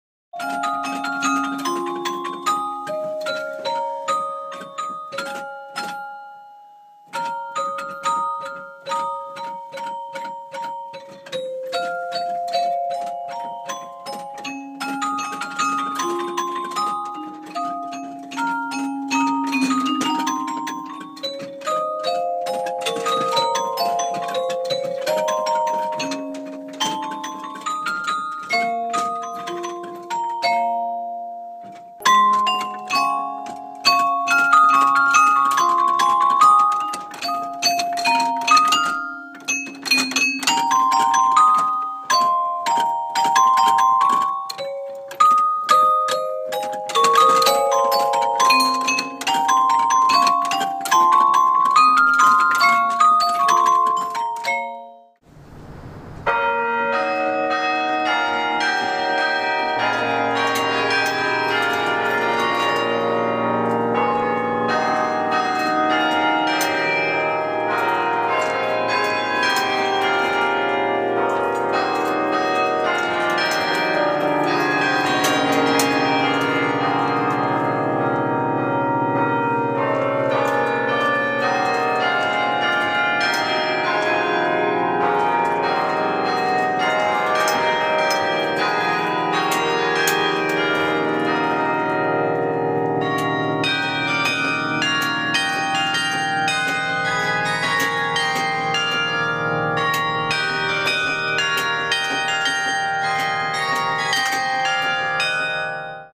Carillon at the National Cathedral
Ed Nassor, the carillonneur at the National Cathedral in Washington, D.C. plays both the practice carillon in his office (a modified xylophone) and the actual 64-ton carillon in the cathedral's central tower on Sunday, February 19th, 2012.